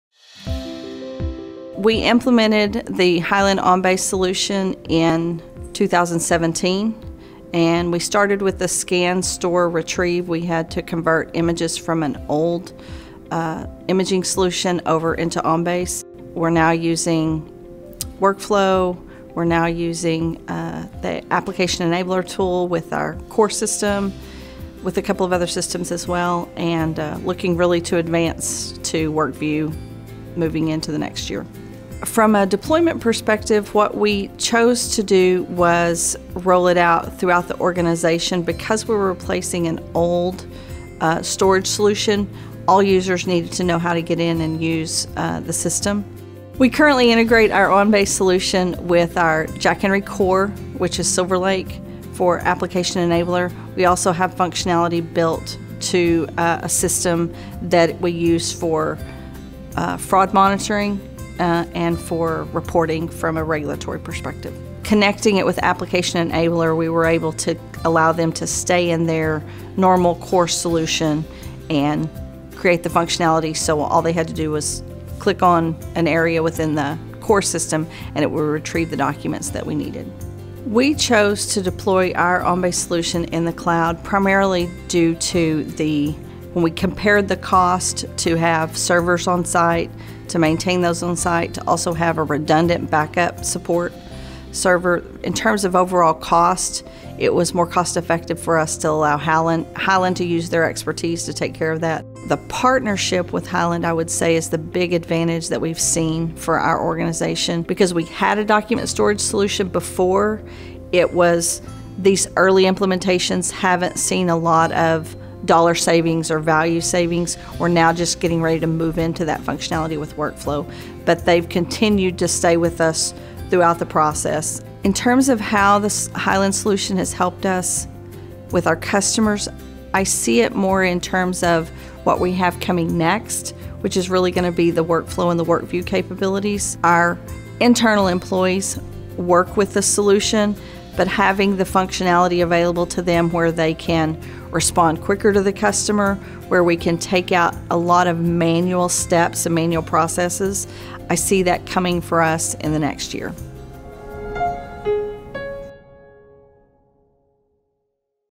American National Bank of Texas Customer Story
The American National Bank of Texas decided to replace their old storage solution with OnBase. Learn how seamless integration with their core system drives value throughout the organization.